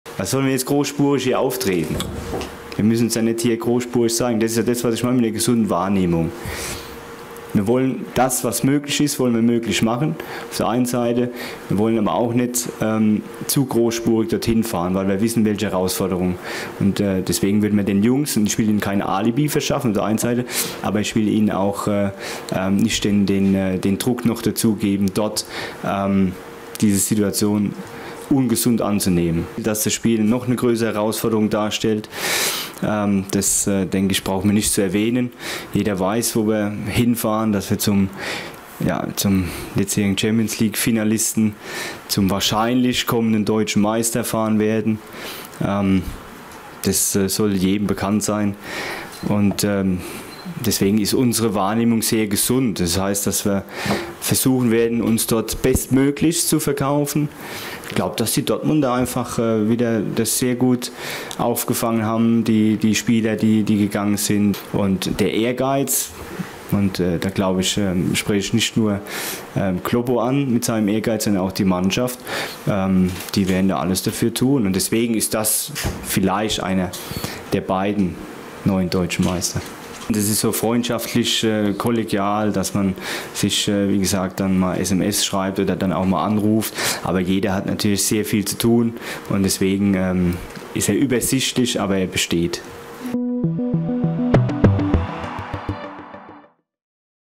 Lieberknecht vor BVB: "Warum sollen wir großspurig auftreten?"
Am Sonntagabend muss Aufsteiger Eintracht Braunschweig beim BVB zum zweiten Spieltag der neuen Bundesliga-Saison antreten. Trainer Thorsten Lieberknecht freut sich auf die Herausforderung.